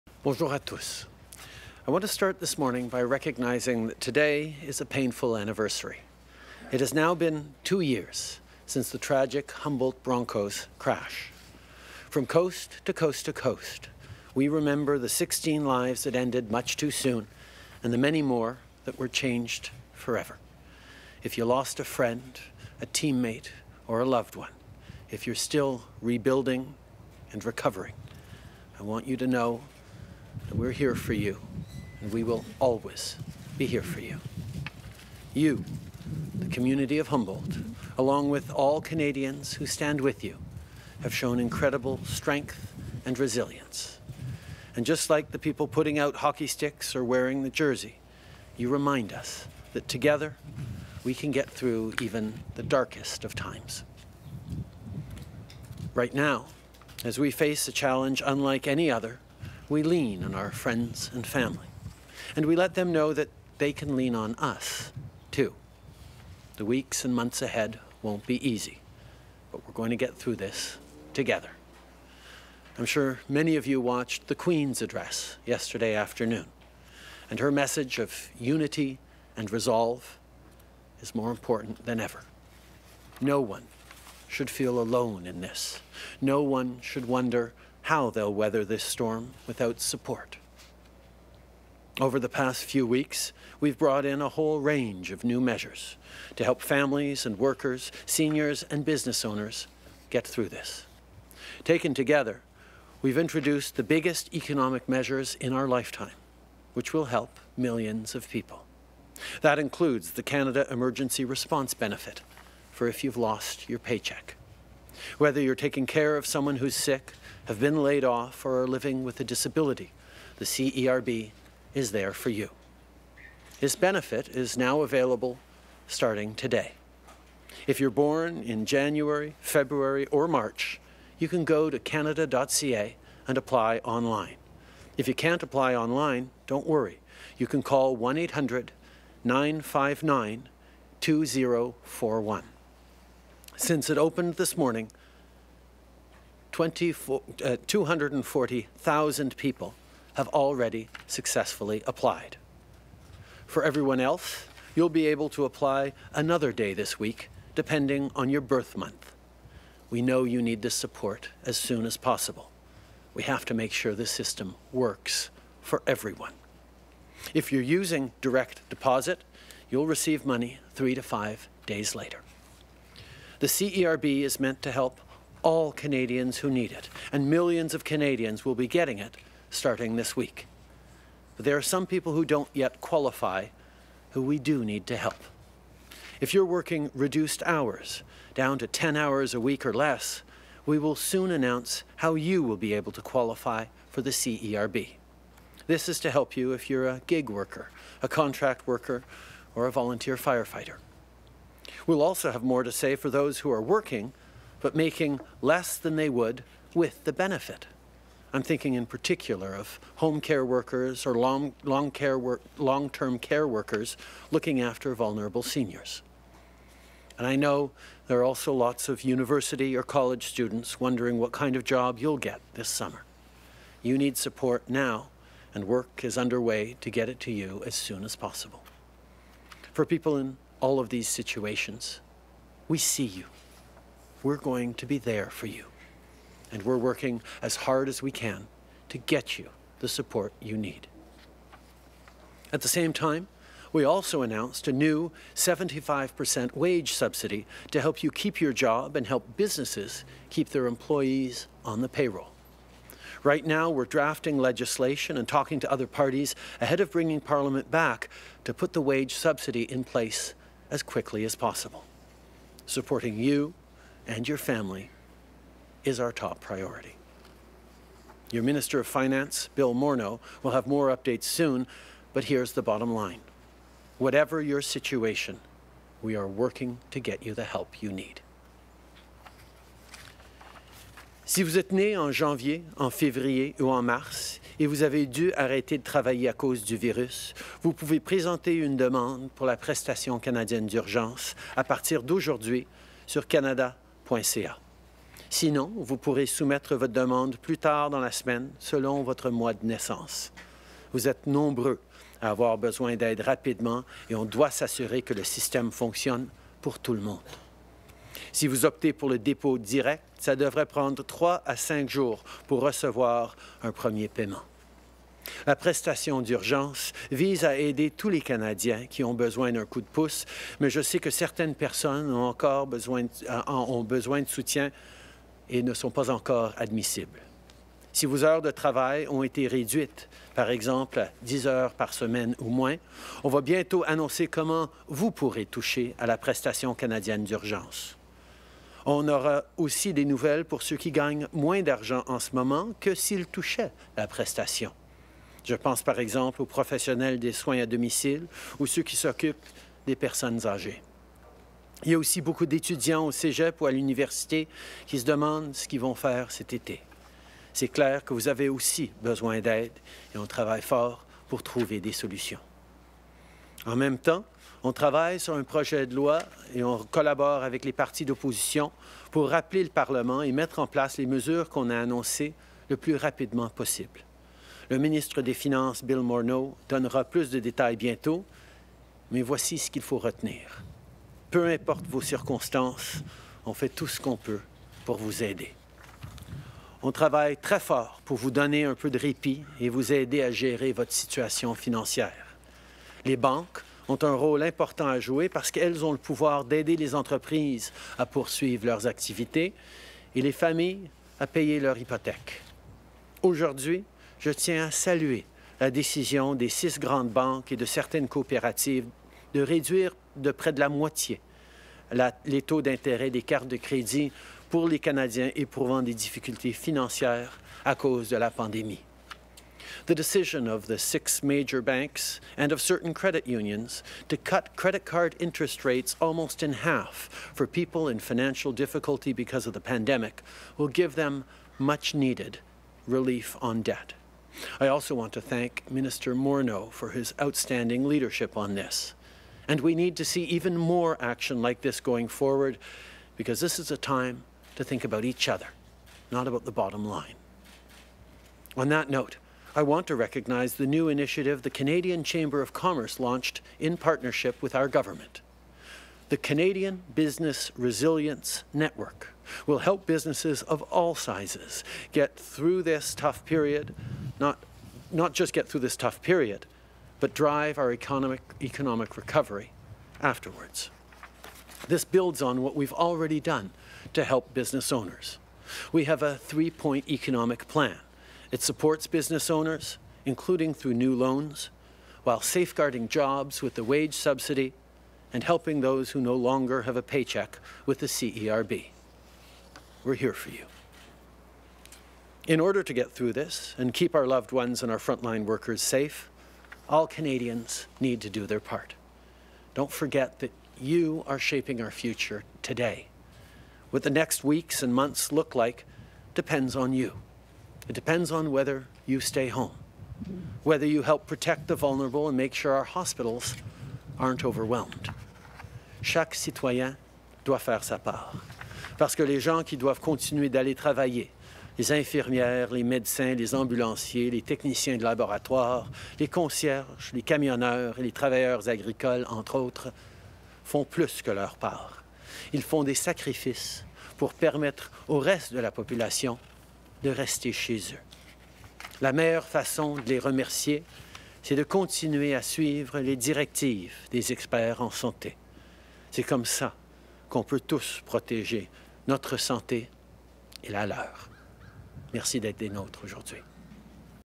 Remarks on the CERB and additional measures to fight economic impacts of COVID-19
Prime Minister Justin Trudeau delivers remarks on the Canada Emergency Response Benefit and additional measures to fight economic impacts of COVID-19.